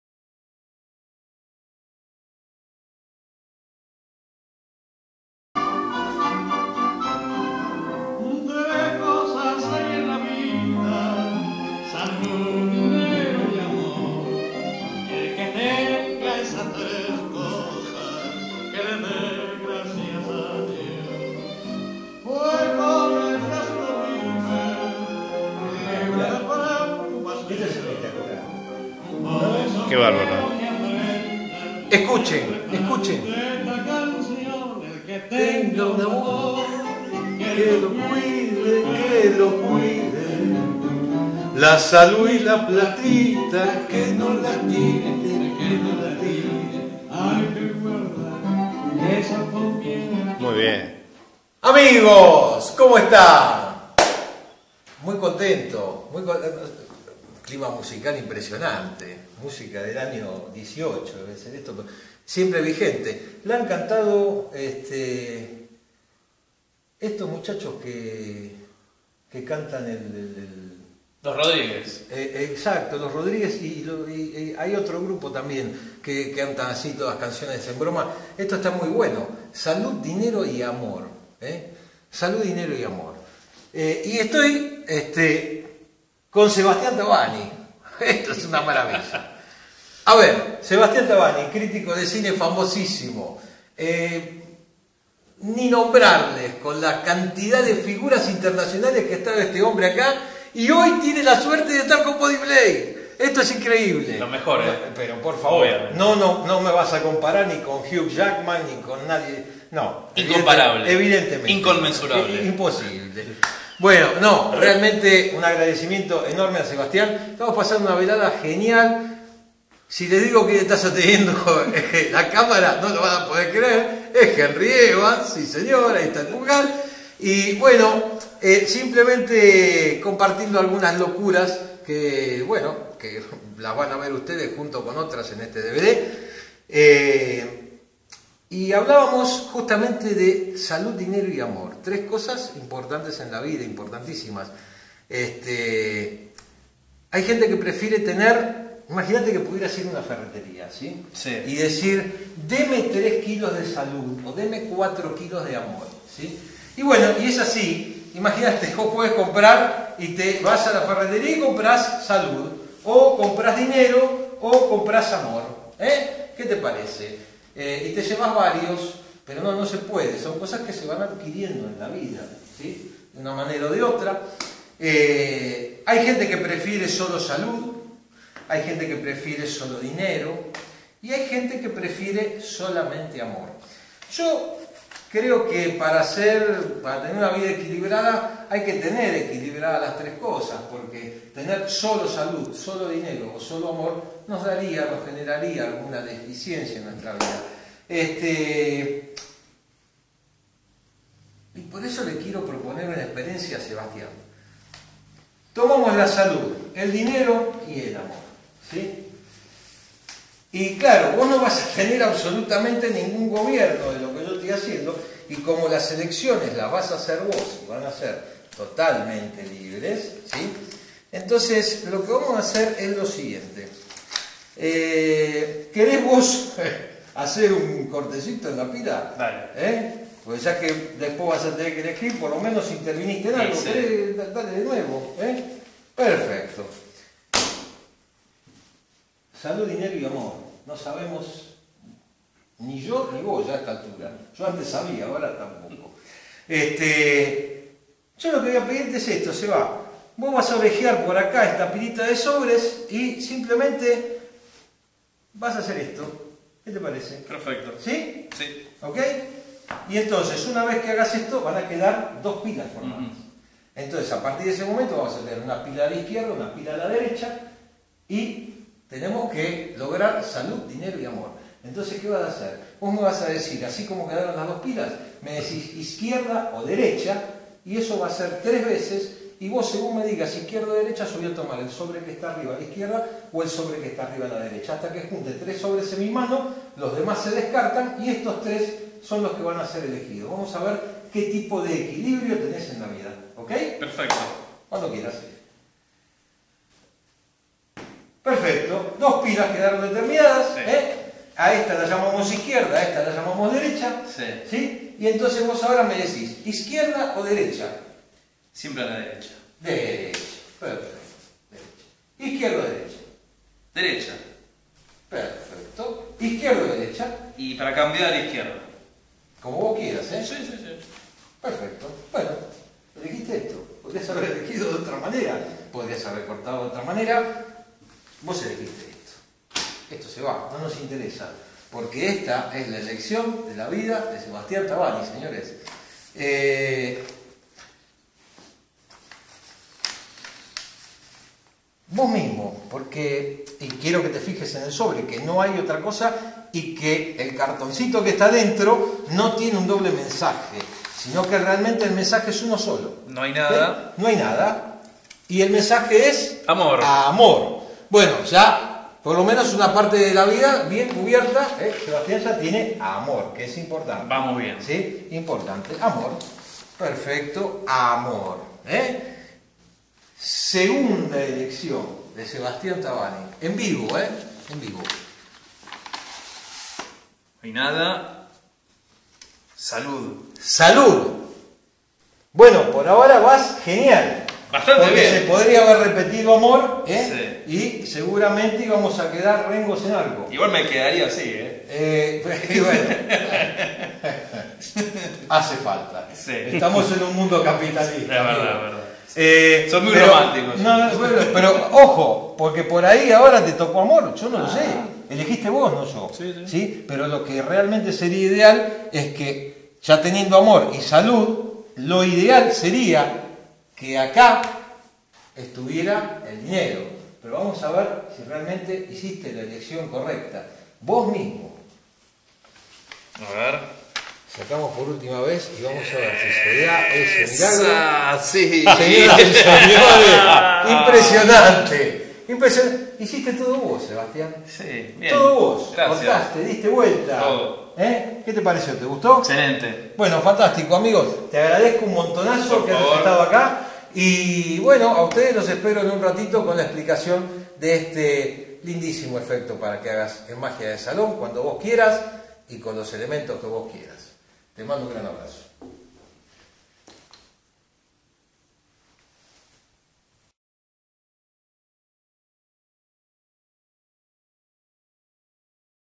SALUD, DINERO Y AMOR por Bodie Blake
Uno de los efectos mis últimos dvds. Más magia simple y 100% efectiva.